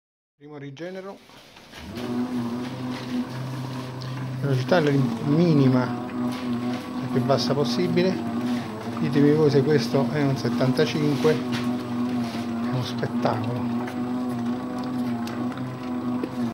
Come impastare 18 kg con la Spirale - 4° video chiusura impasto
Impasto 75% di idratazione
Polselli Super
Farina 5.000
Acqua 3.750
Sale 100
Ldb 20

Per chiudere l'impasto fate uno stop di 10 minuti e poi avviate la macchina a bassa velocità per un singolo giro di vasca.
Ripetere due volte.